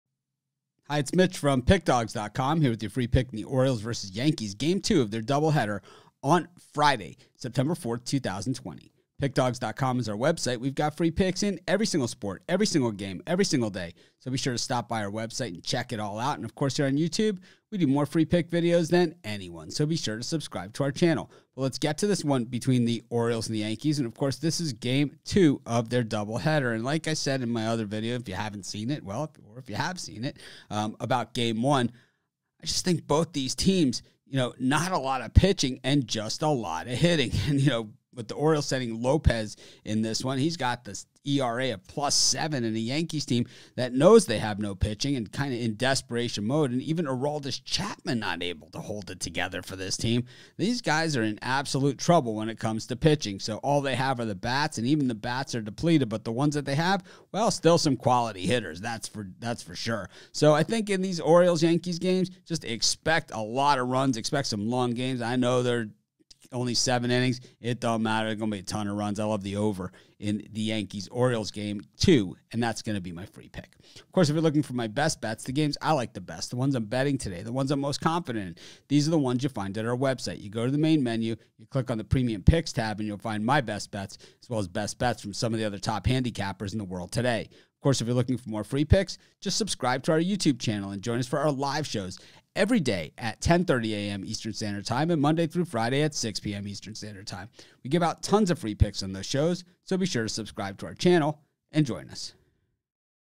New York Yankees vs Baltimore Orioles Game 2 Free Pick 9/4/20 MLB Pick and Prediction MLB Tips
The Baltimore Orioles face off with the New York Yankees in game two of a two-part series in Friday's MLB Baseball action. Get Mitch's Expert Pick and Prediction for this game.